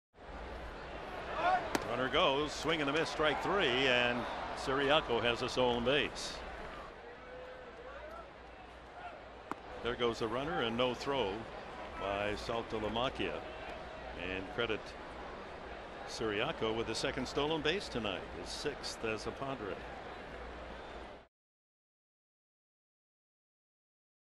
2013/07/02 Ciriaco swipes two bags
SD@BOS: Ciriaco swipes two bags at Fenway